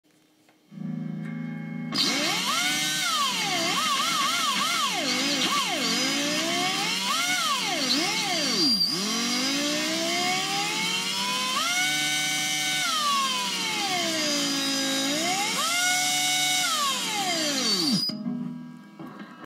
Power Drill and a Guitar
What happens when you put a power drill next to a guitar pickup! Made famous by Eddie Van Halen.